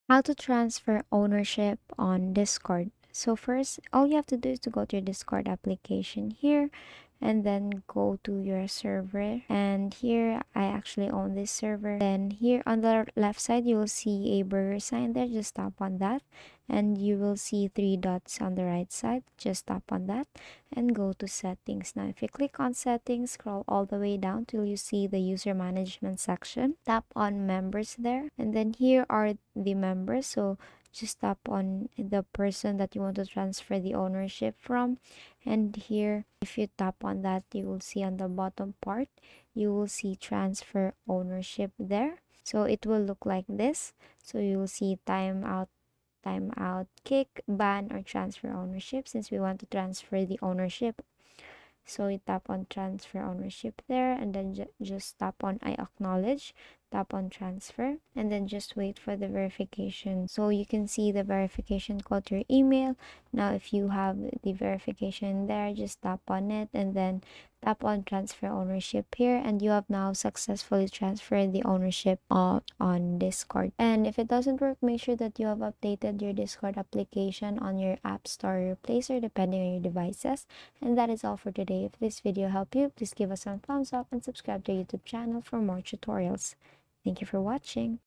How to Transfer Ownership on Discord (2023)
Learn‎‎‏‏‎ ‎How to Transfer Ownership on Discord

In this video I will show you‏‏‎ how to transfer ownership on discord‎